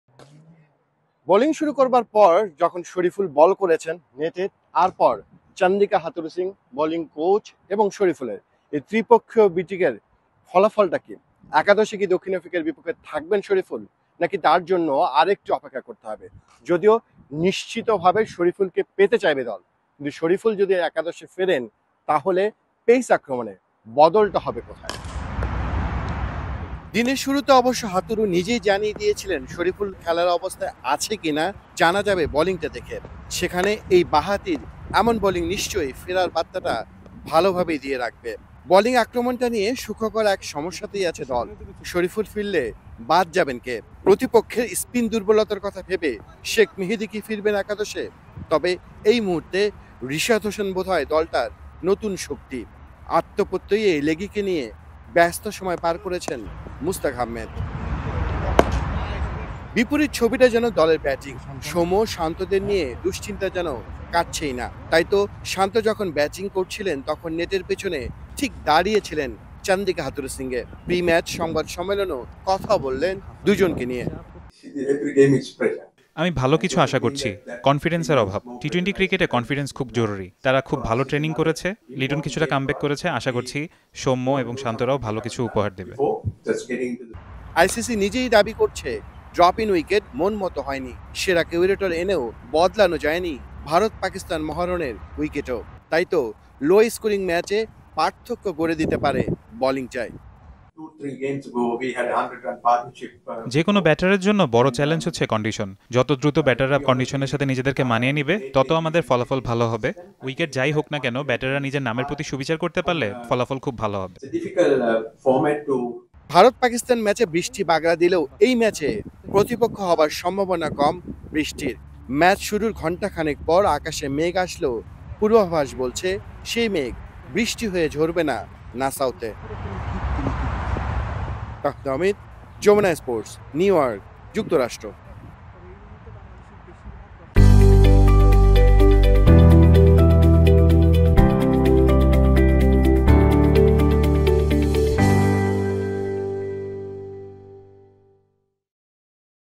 বাংলাদেশ-দ.আফ্রিকা ম্যাচে কেমন হবে উইকেটের আচরণ, খেলবেন শরিফুল? | BD vs SA | Jamuna Sports
কেমন হবে নাসাউ কাউন্টি স্টেডিয়ামের ড্রপ ইন উইকেটের আচরণ? আরো একটি লো স্কোরিং ম্যাচেই কি হতে যাচ্ছে? উইনিং কম্বিনেশন কি ভাঙবে বাংলাদেশ; পেসার শরিফুল ইসলাম কি খেলবেন?

Address:
Bangladesh.